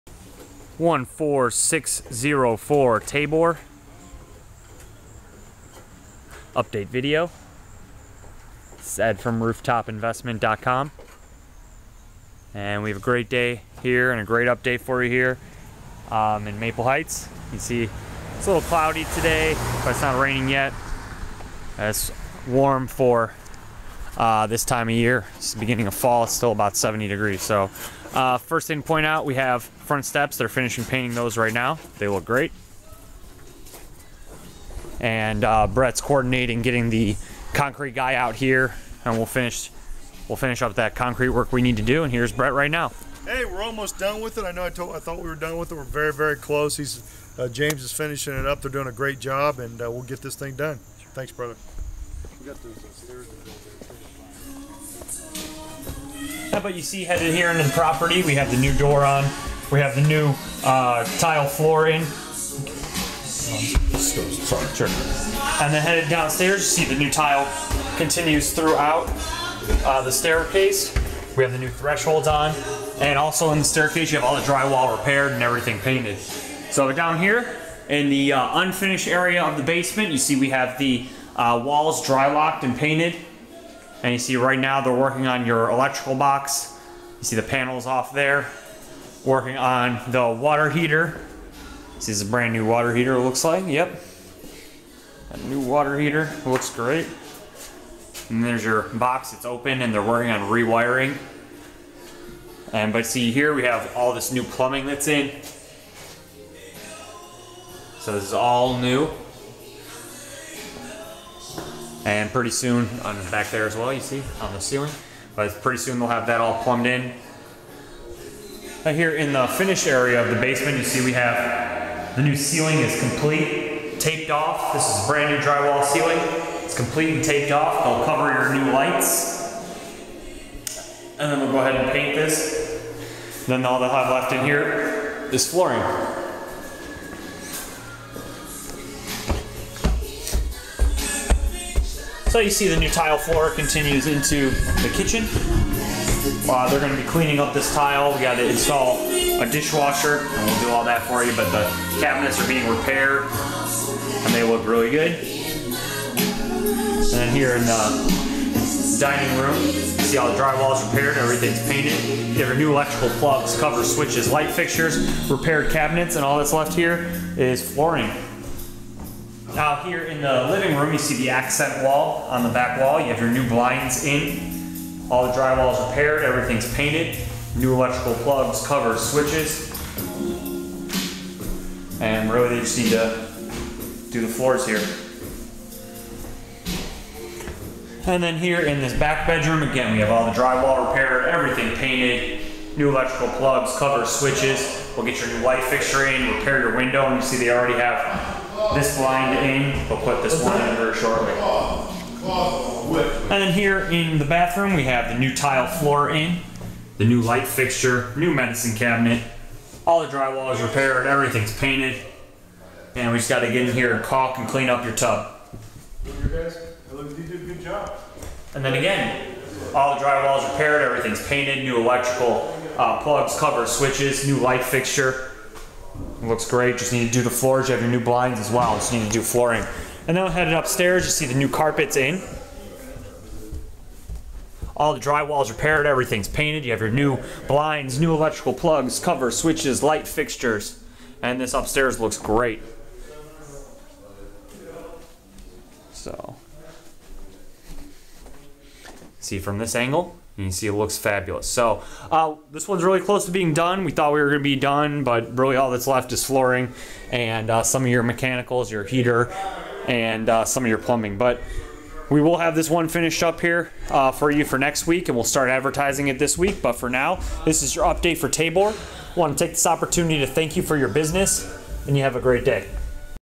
14604 Tabor Update 3
This is a large 3BR 1BA single family home in Maple Heights Ohio. This home is being completely remodeled, updates include NEW DISHWASHERS, new flooring/carpet, fresh paint, new electrical plugs covers switches light fixtures. Too many updates to list.